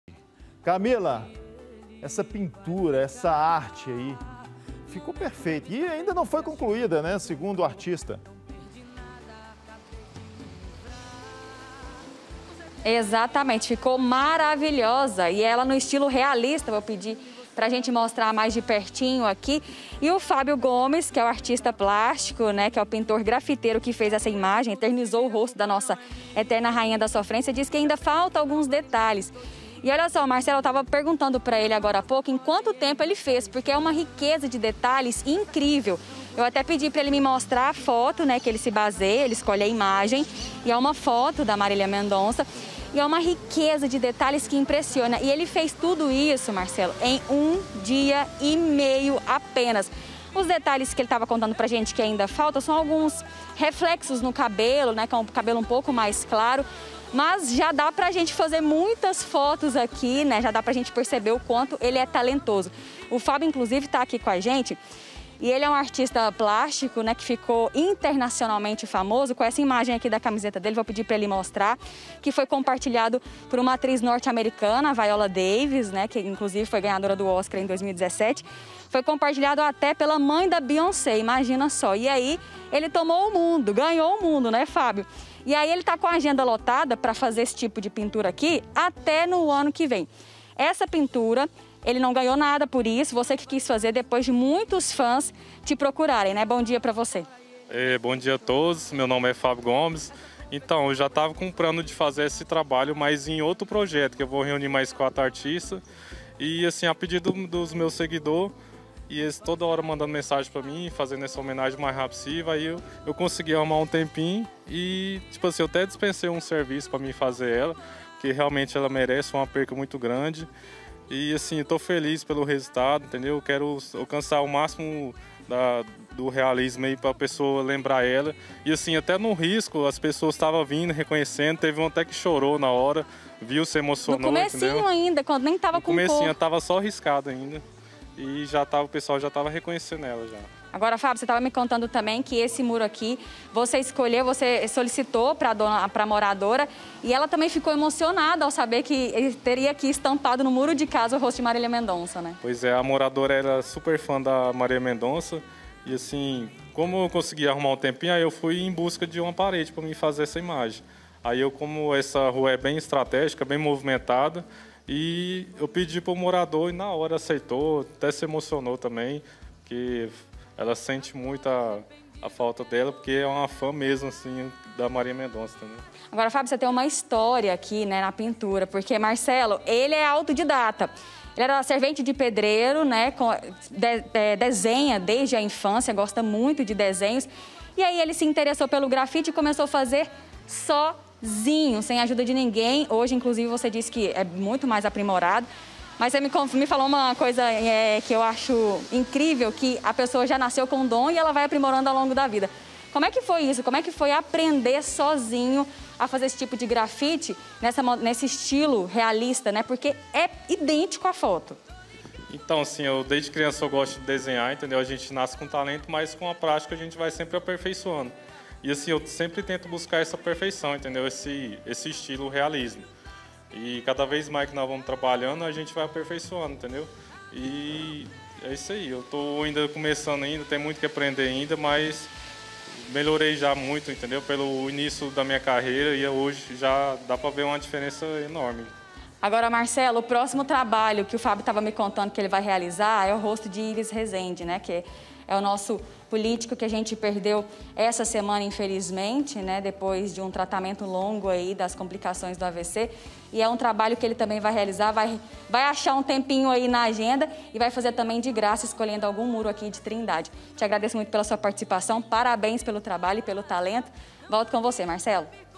HOMENAGEM: ARTISTA PINTA ROSTO DE MARÍLIA MENDONÇA EM MURO
E A KAMYLLA AGORA VAI MOSTRAR PRA GENTE UMA HOMENAGEM QUE A CANTORA MARÍLIA MENDONÇA RECEBEU EM TRINDADE.